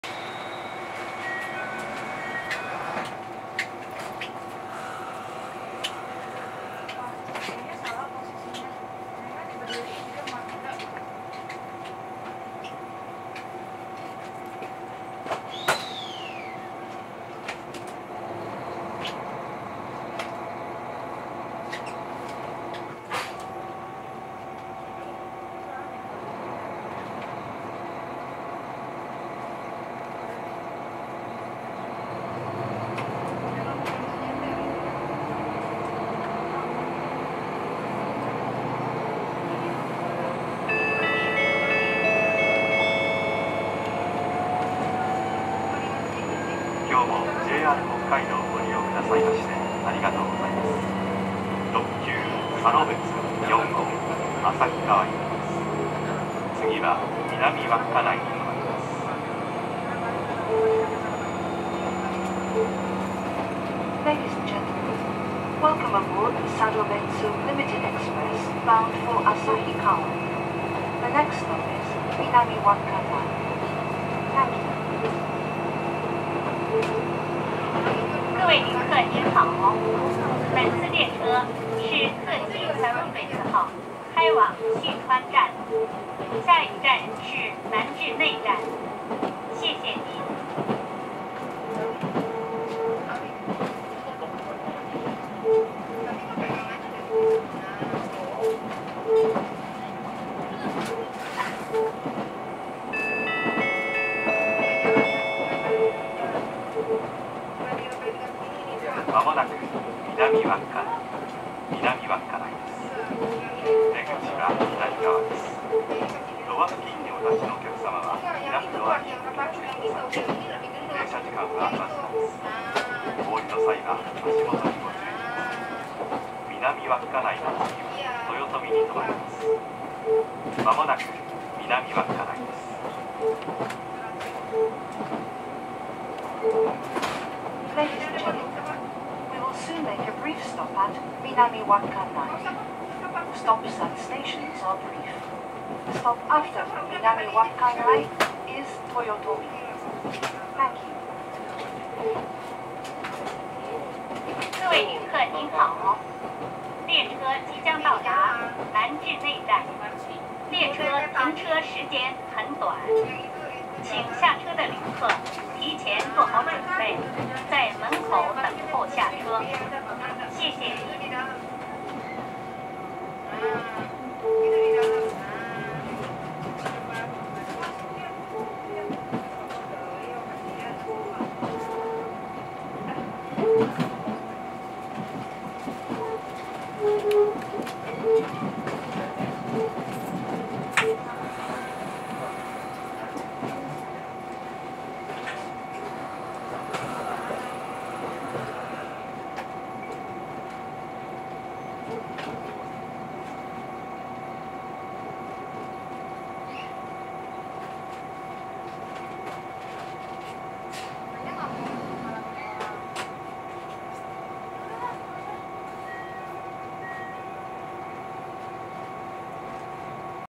北海道鉄道動画（稚内市）4K対応その2 Videos taken in Hokkaido
ＪＲ宗谷本線の稚内駅から南稚内駅までの鉄道動画です。２０２３年４月３０日撮影。
Take the train from Wakkanai Station to Minami-Wakkanai Station